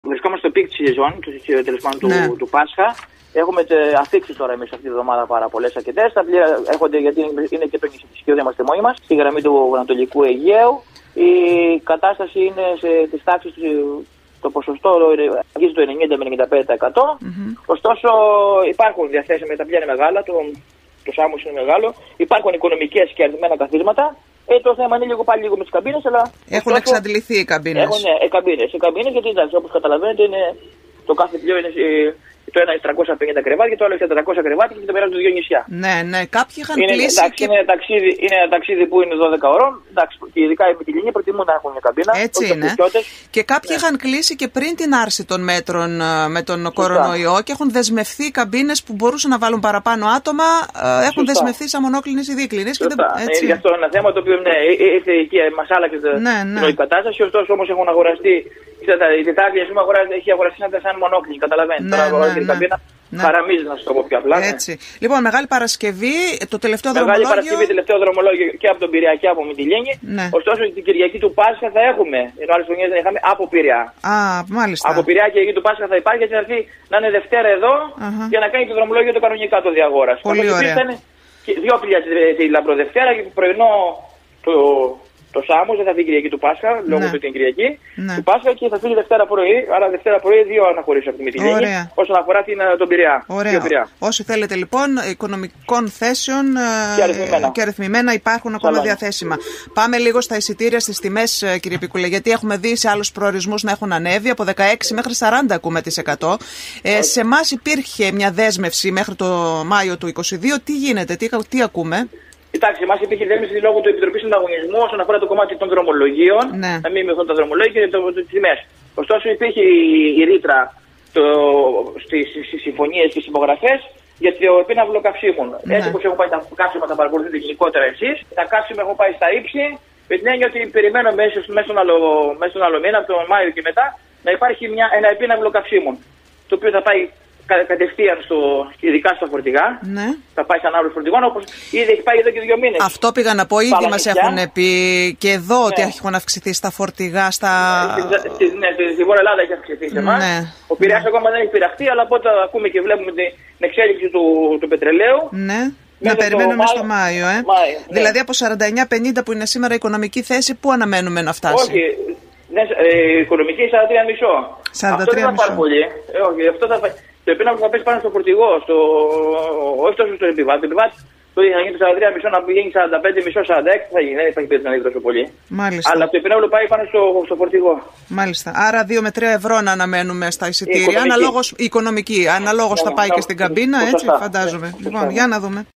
Αύξηση ακτοπλοϊκών εισιτηρίων το Μάιο για τη Λέσβο
Ηλίας Πίκουλος-Κεντρικός ναυτιλιακός πράκτορας Λέσβου

Ρεπορτάζ: Μυρσίνη Τζινέλλη